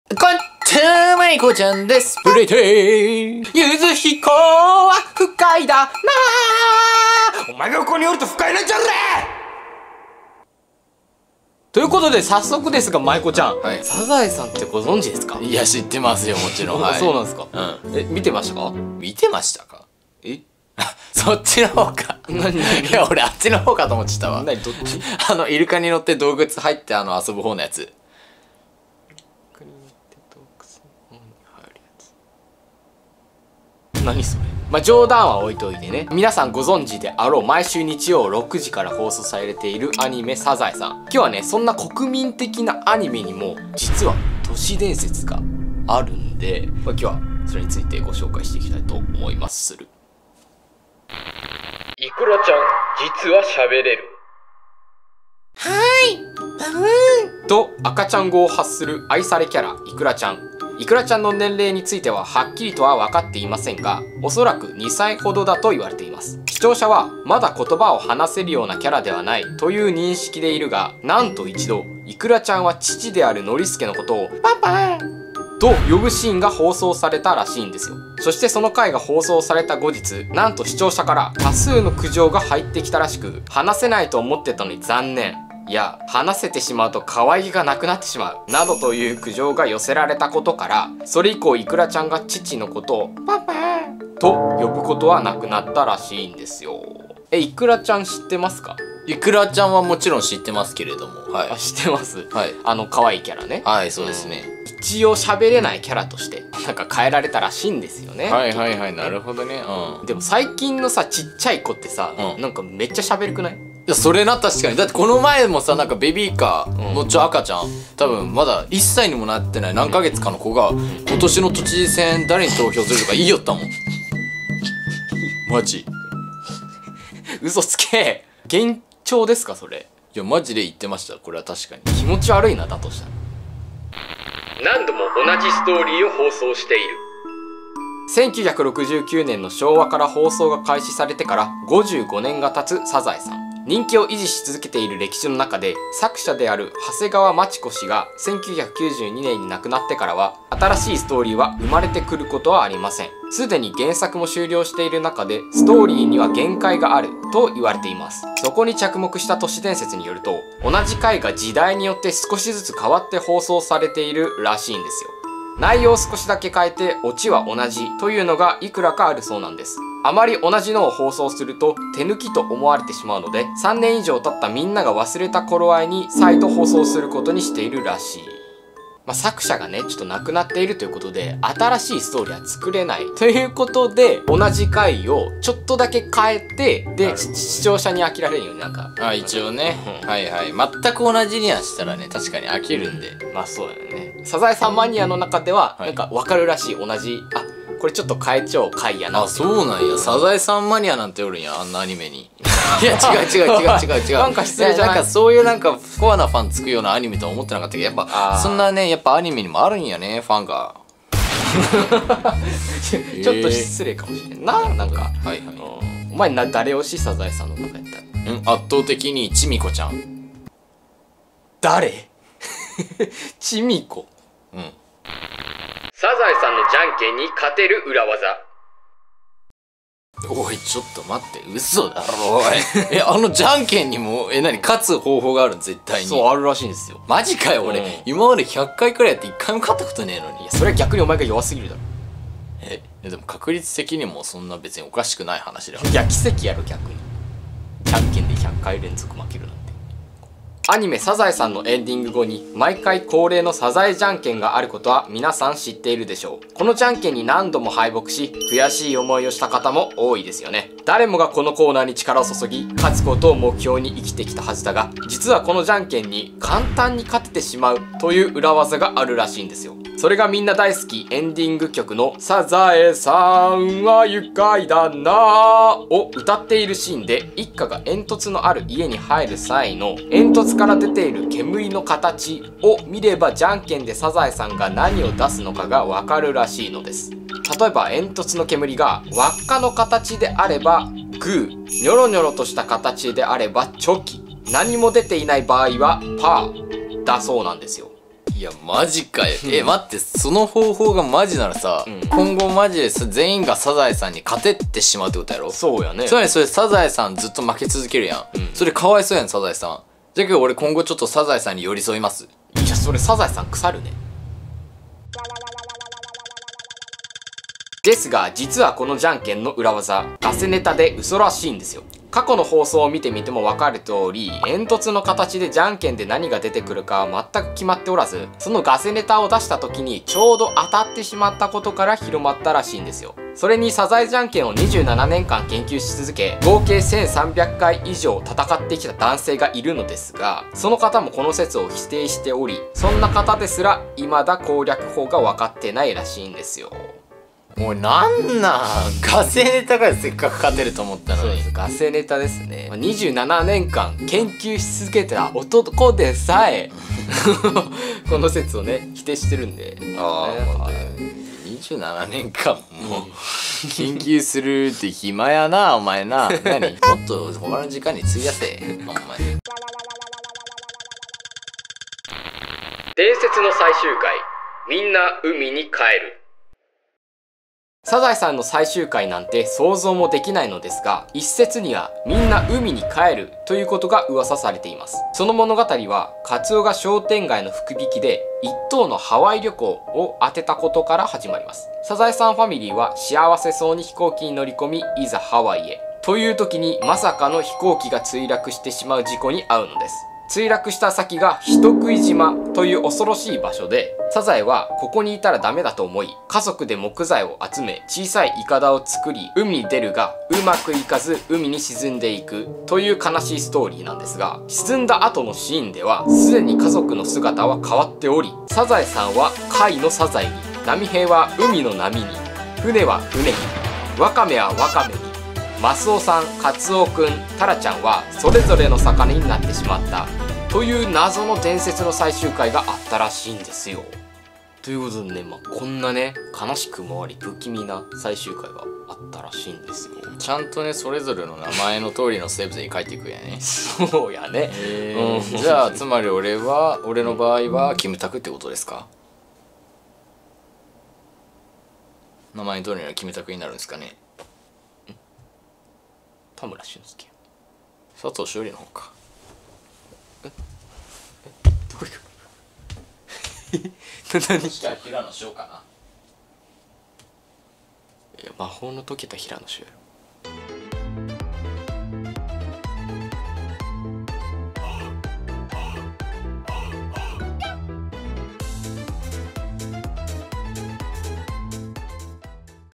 【恐怖】サザエさんの都市伝説が怖すぎた...😱
追記：腹へった
編集：(柚彦)

ファンレター・プレゼントの送り先はこちら↓
〒150-0002
株式会社BitStar  マイコちゃん[MAIKO]宛